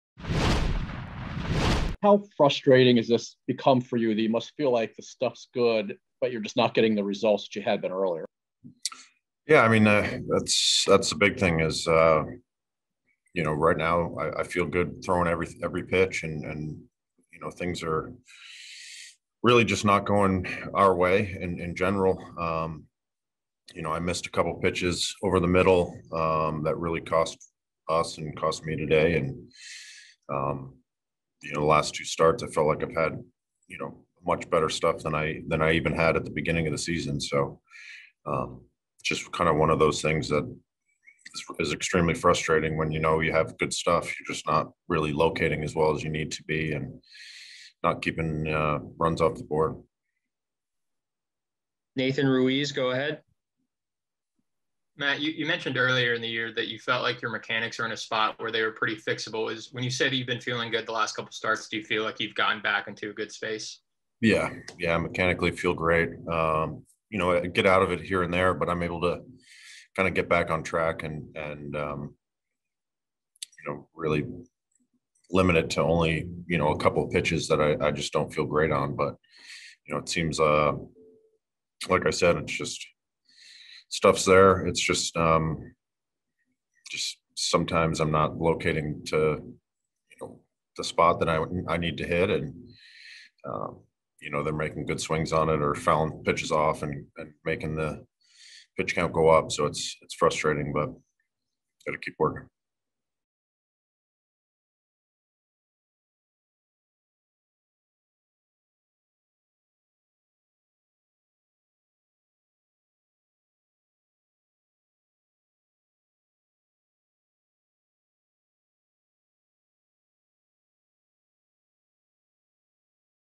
Matt Harvey on "frustrating" start against White Sox
Matt Harvey struck out six and allowed five runs in three innings on Saturday. More